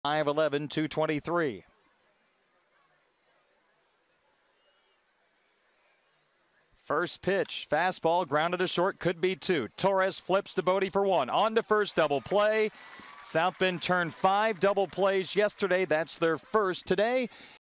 Cubs' Hedges induces a double play
7/20/15: South Bend's Zach Hedges escapes trouble in the fourth with a double play against West Michigan